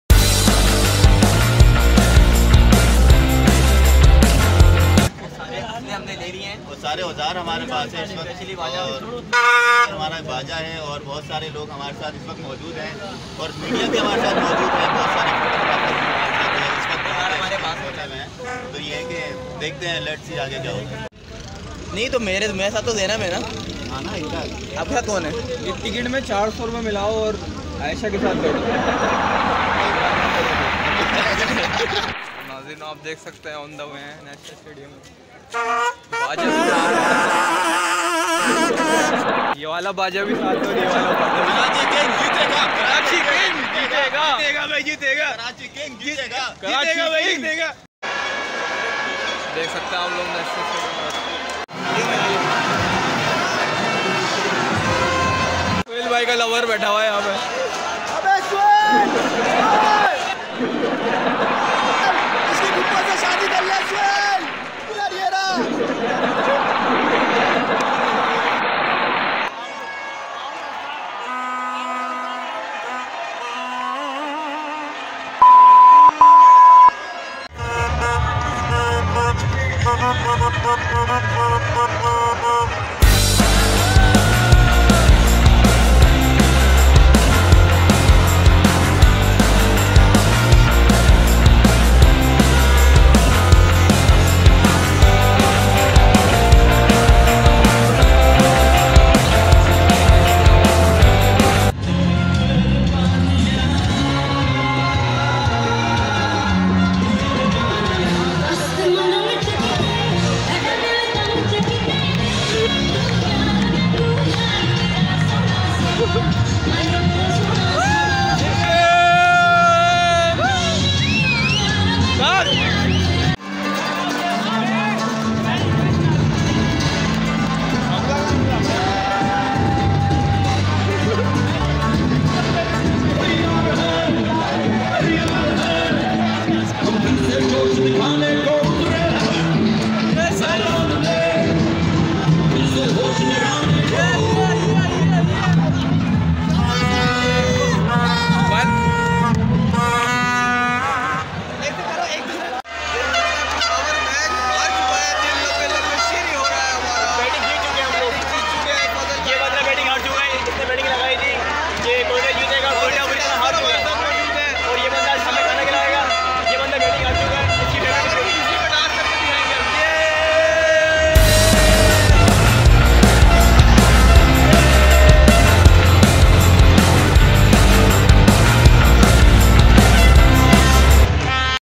Fun in national stadium karachi | PSL vlogs | HblPsl5 2020 | syed Ahmed raza
Making my first vlog in national stadium karachi .  quetta v Peshawar match held on 22 march 2020 Pakistan super League came in 4 city's of Pakistan  in this you can enjoy many funny moments which was recorded during the match